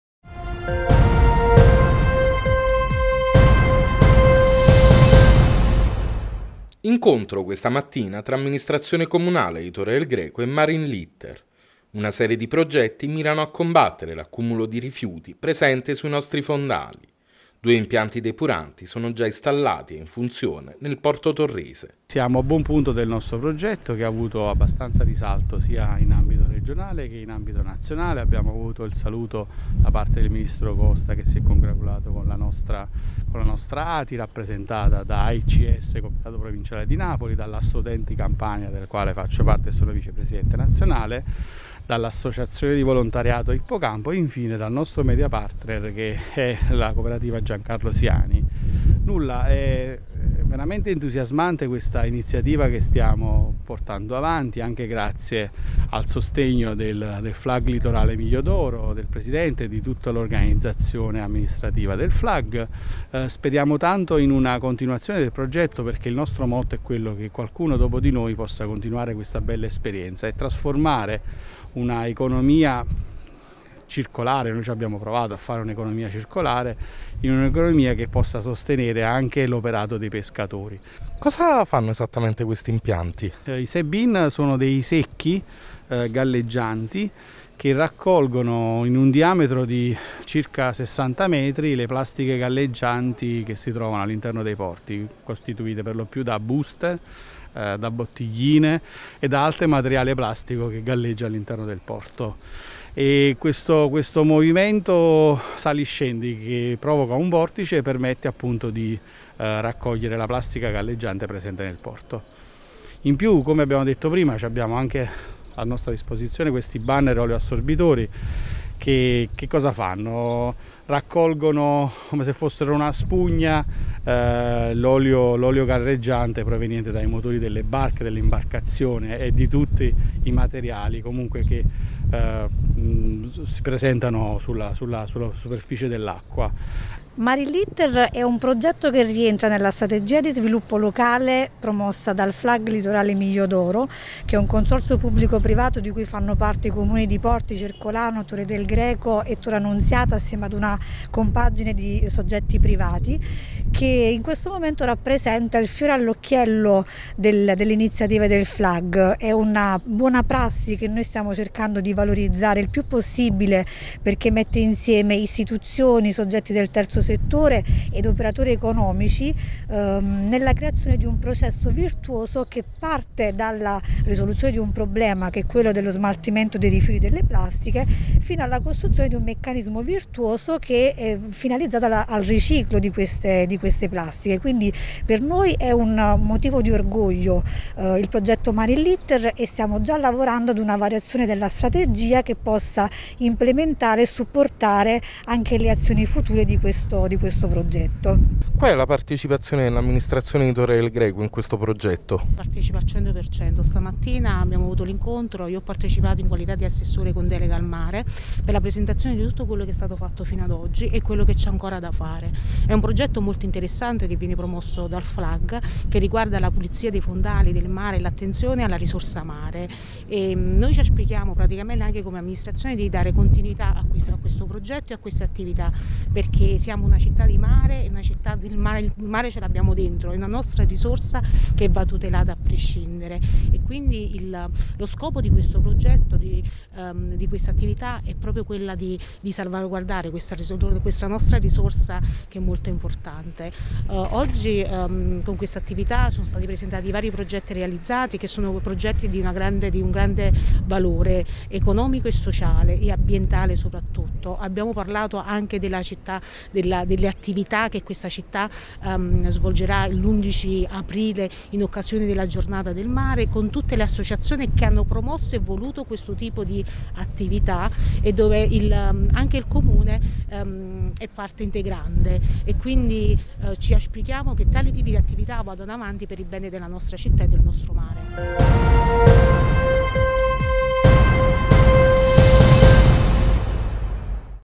'Marine Litter': incontro con il comune per la salvaguardia dei fondali marini
Incontro tra amministrazione comunale di Torre del Greco e Marine Litter. Una serie di progetti mirano a combattere l’accumulo di rifiuti presente sui nostri fondali. Due impianti depuranti sono già installati e in funzione nel porto torrese.